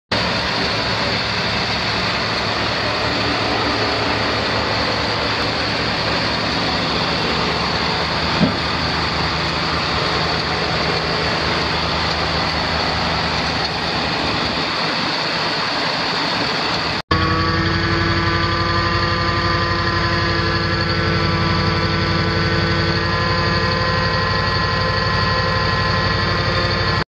1980 GMC 7000 oil distributor truck for sale at auction | bidding closes February 8, 2022
1980 GMC 7000 oil distributor truck
Miles: 32,198 on odometer
Hours: 1,960 on meter
VIN: T17DBAV605494
Engine
Cylinders: 8
Fuel type: Gas
Transmission
Manual
Speed: 5, plus 2
Chassis
Axles: Single
Suspension: Spring
Brakes: Hydraulic
Interior
Heat
Operational
Bed / tank
Etnyre Black Topper
Serial: J8876
Capacity: 1,074 gallons
9' L x 78" W
Oil distributor
Burner: LPG
In-cab controls
Spray wand
Spray boom
Width: 13'
Folding
Nozzles: 44

Tires
Size: 10.00-20
It's located in Wellington, KS, and we're selling it to the highest bidder regardless of price.